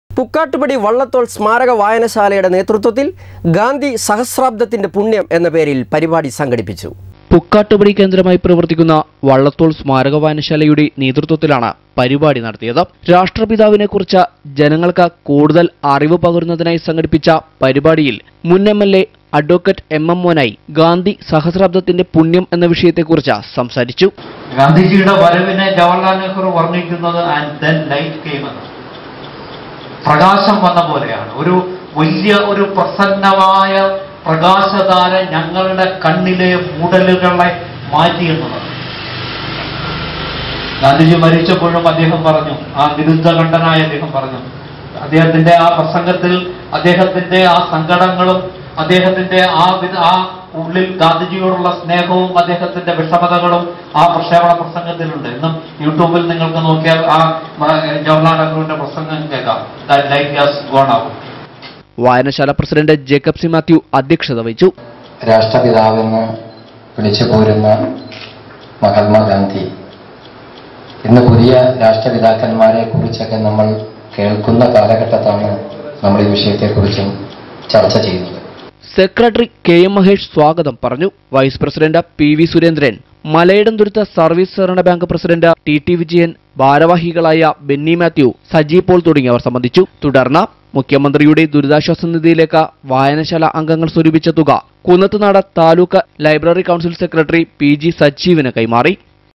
ഗാന്ധി സഹസ്രാബ്ദത്തിന്റെ പുണ്ണ്യം എന്ന പേരിൽ പരിപാടി സംഘടിപ്പിച്ചു.
പൂക്കാട്ടുപടി വള്ളത്തോൾ സ്മാരക വായന ശാലയുടെ നേതൃത്വത്തിൽ ഗാന്ധി സഹസ്രാബ്ദത്തിന്റെ പുണ്ണ്യം എന്ന പേരിൽ പരിപാടി സംഘടിപ്പിച്ചു.